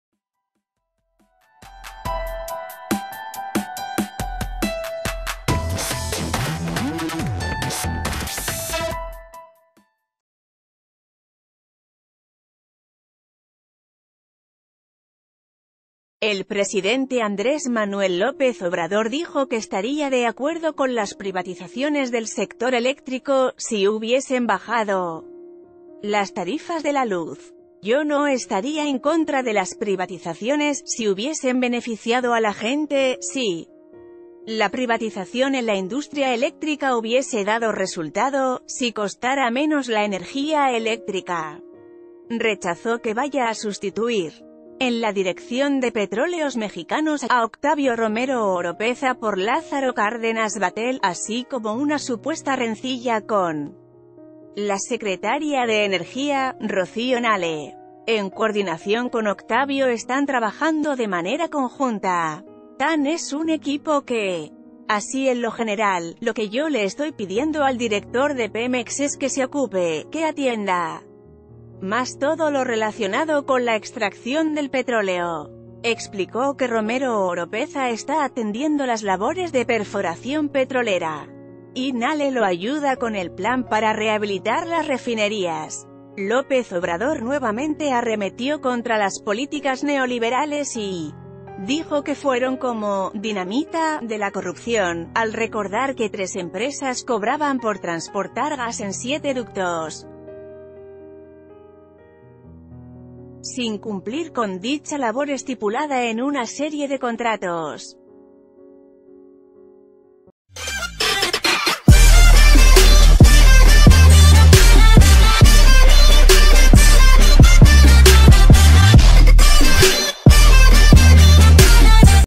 Yo no estaría en contra de las privatizaciones, si hubiesen beneficiado a la gente: AMLO
El presidente Andrés Manuel López Obrador dijo que estaría de acuerdo con las privatizaciones del sector eléctrico, si hubiesen bajado las tarifas de la luz. Yo no estaría en contra de las privatizaciones, si hubiesen beneficiado a la gente, si la privatización en la industria eléctrica hubiese dado...